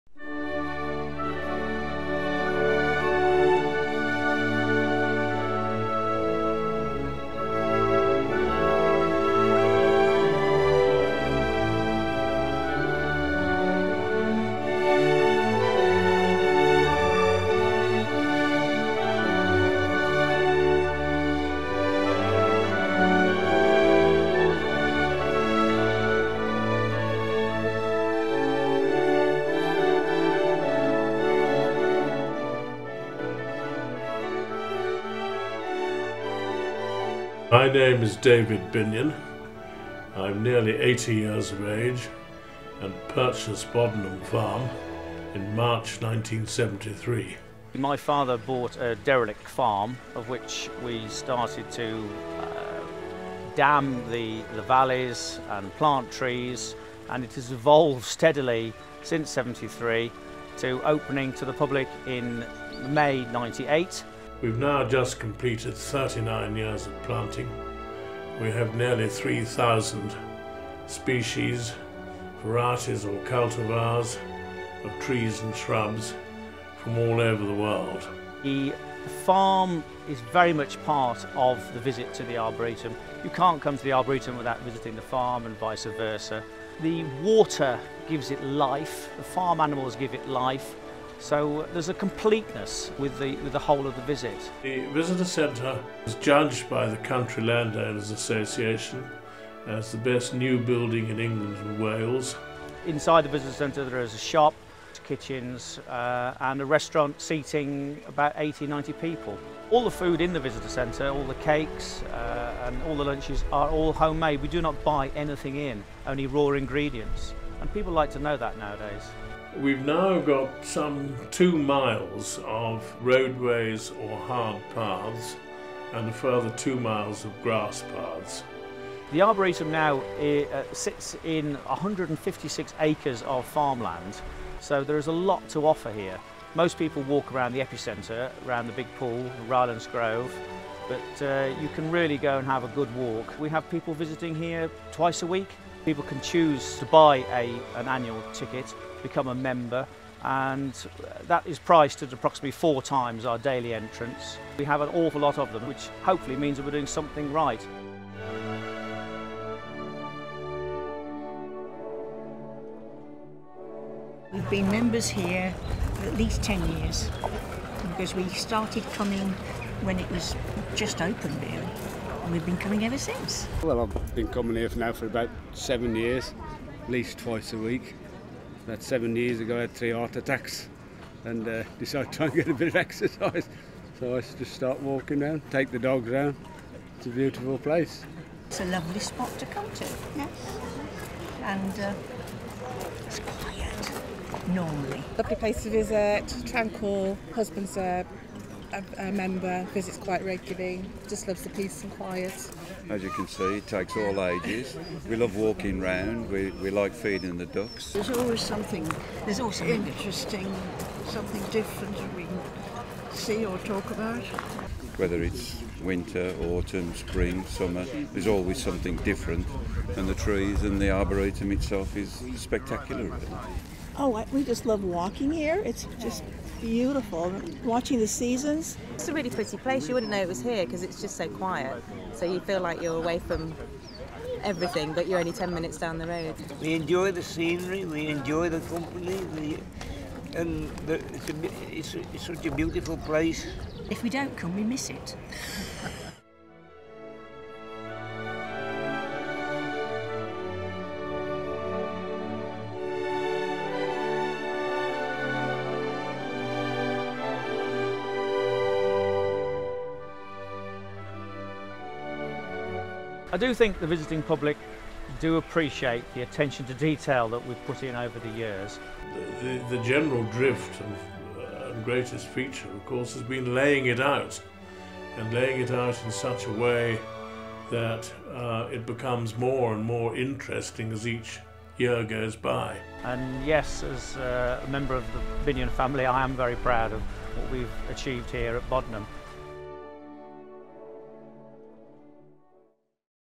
Bodenham Farm and Arboretum
A look at what Bodenham Farm & Arboretum has to offer to visitors.
With thanks to Dewolfe Music